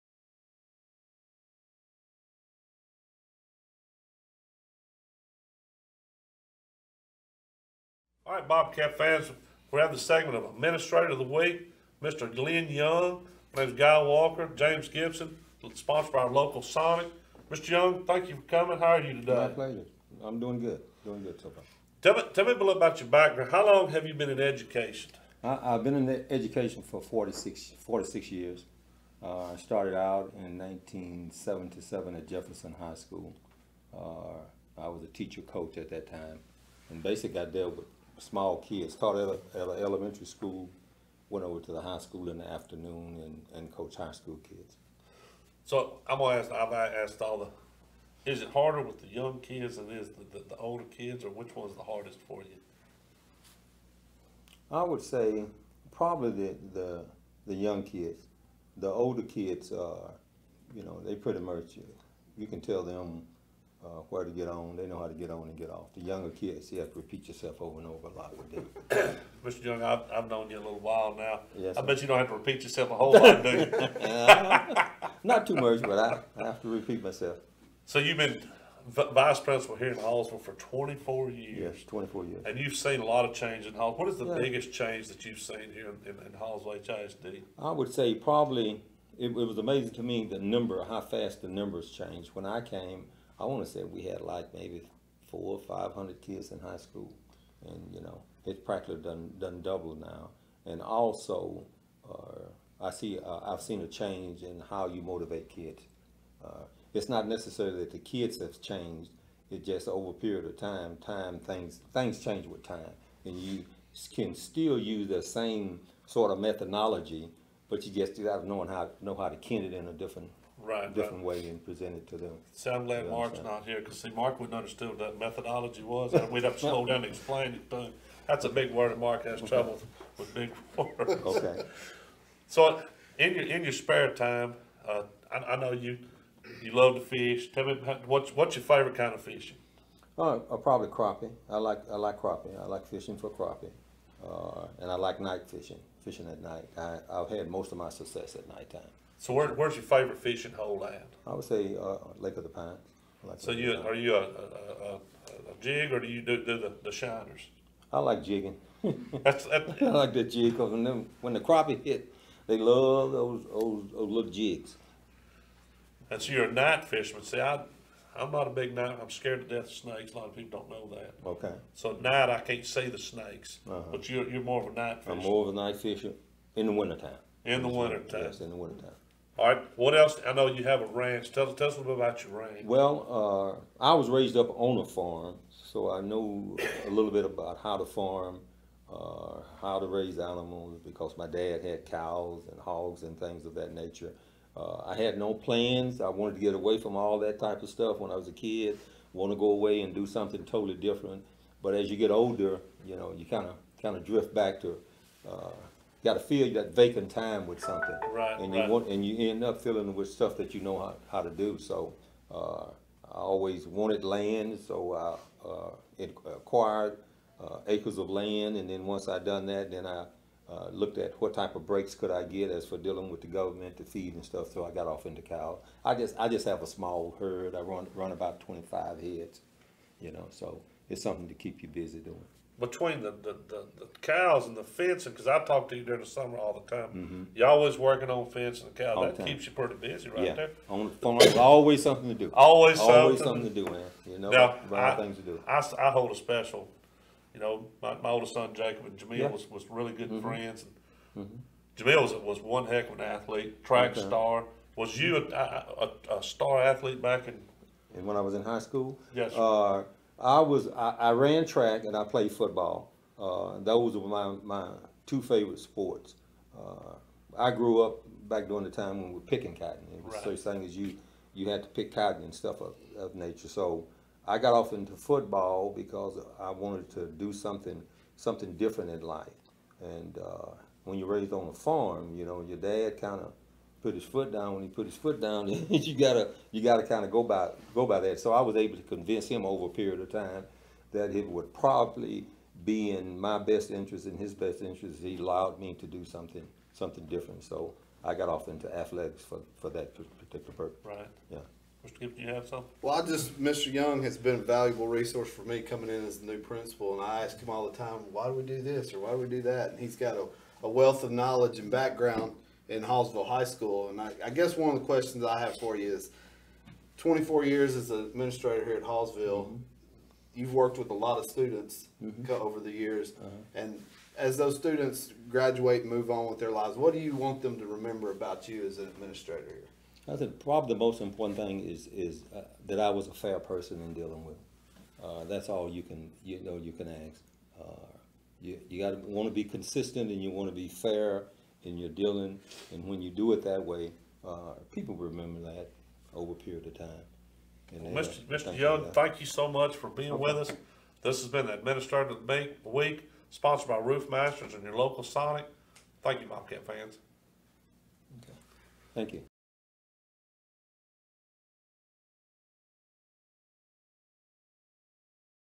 Admin of the Week - Lynn Young - February 2nd, 2022
Guy Walker from the Bobcat Radio Network with special guest James Gibson, HHS Principal, interview Lynn Young, HHS Assistant Principal in this week's segment Admin of the Week brought to you by the following: